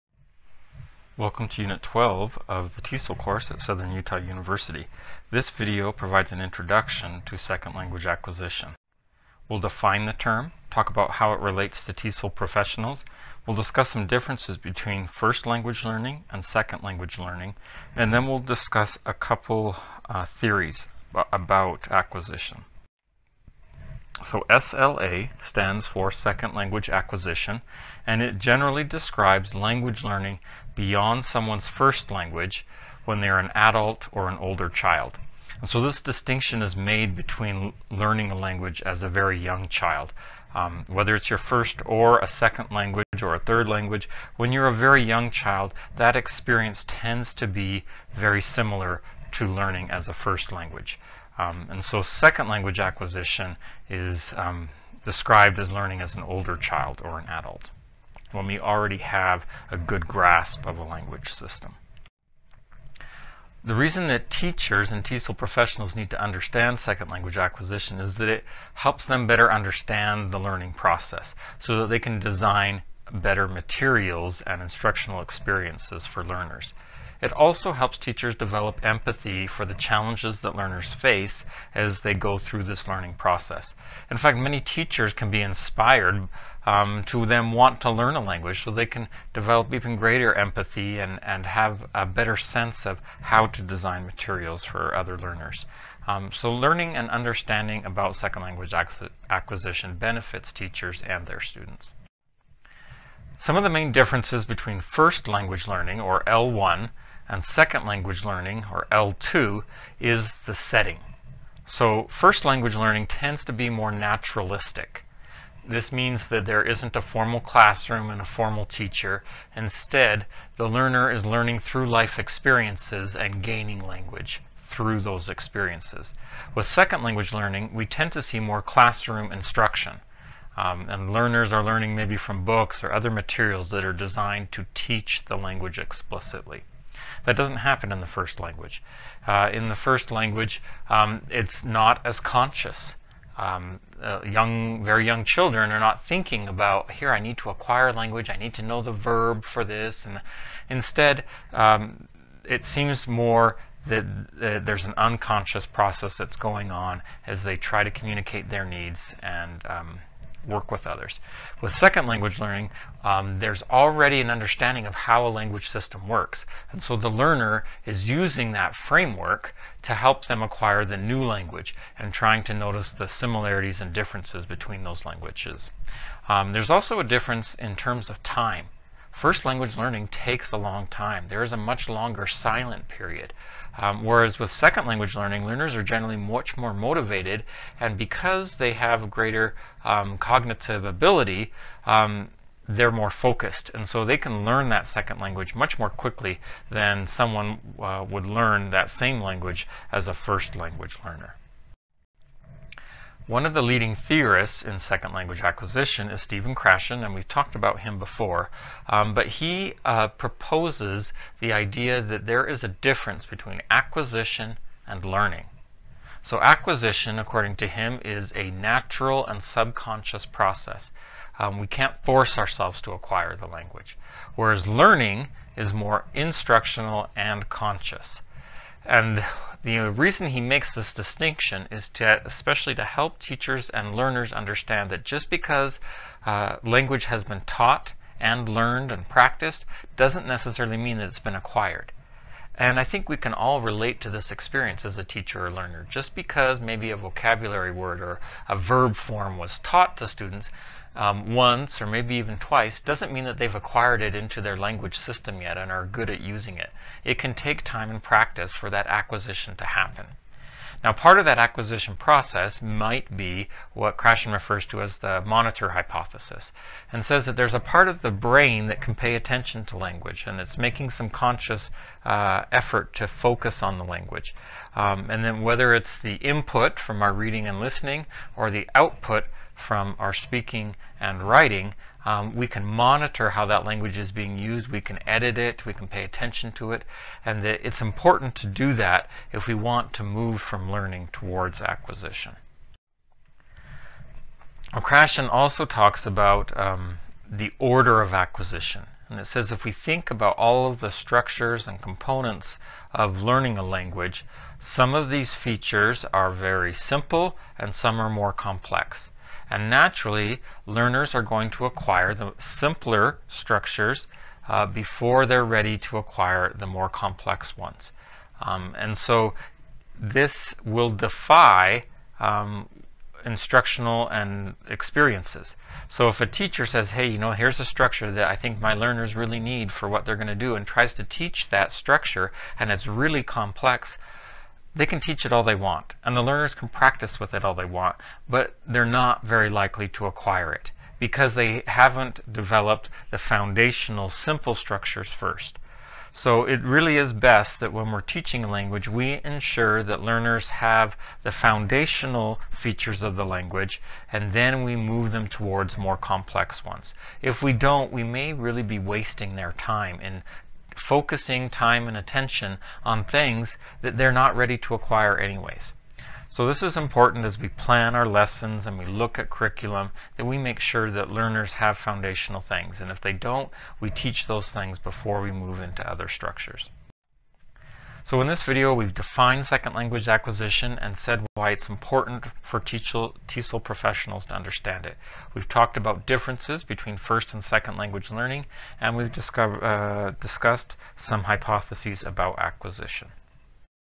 Unit 12 Second Language Acquisition Introduction
This video offers an introduction to theories about second language acquisition (SLA) and why they matter to TESOL professionals. This video was created for the TESOL program at Southern Utah University in Cedar City, Utah, USA.